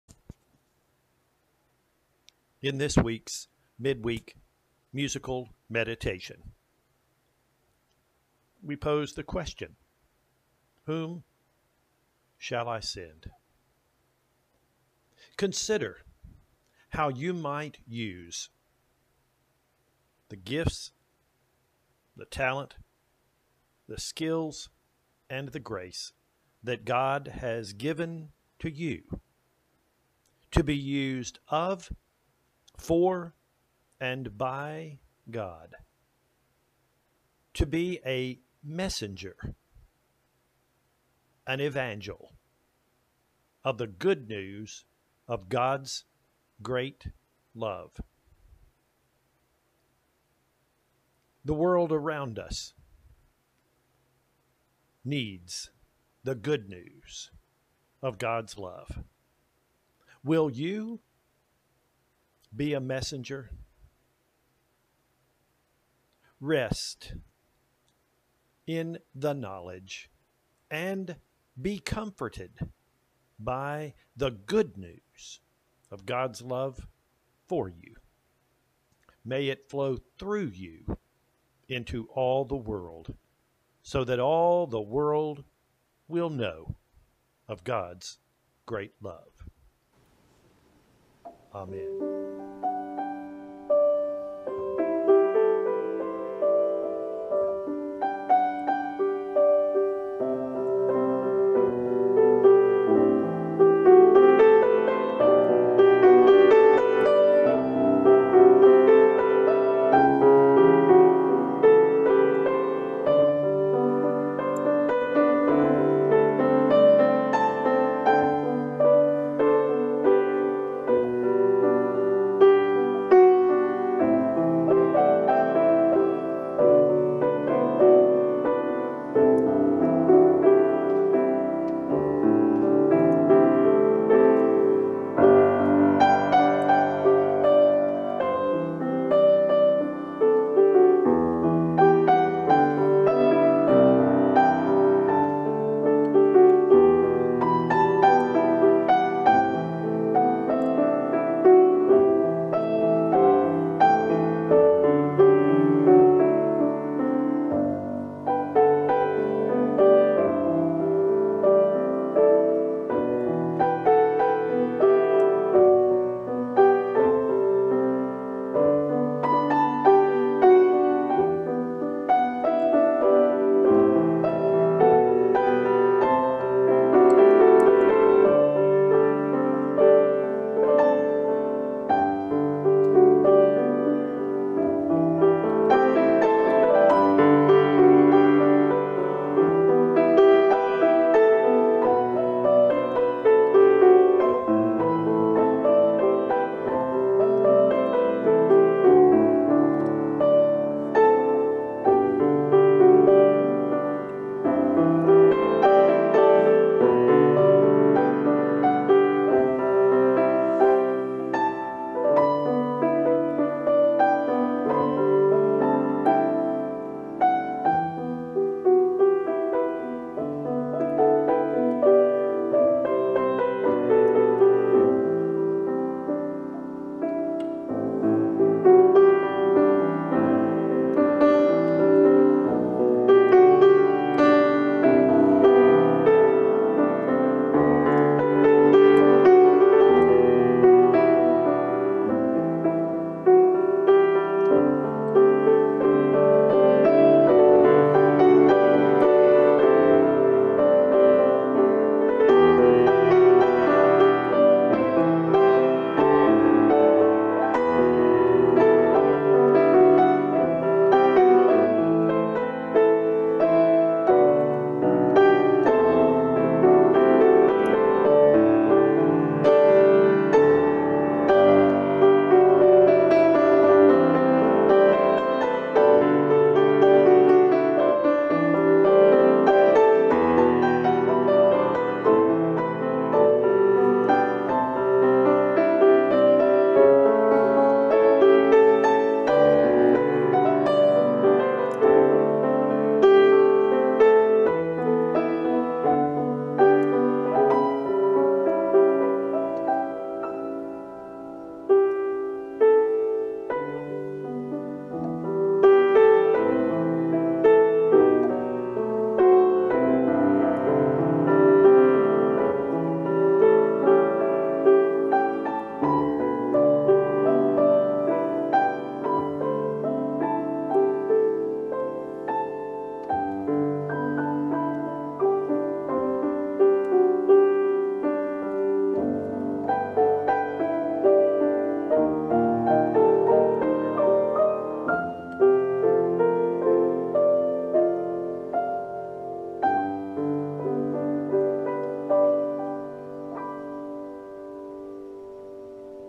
RFUMC Musical Meditation - August 26, 2020
Message by Rev. Dr. Robert C. Brown. Music performed by Julie Bearden-Carver.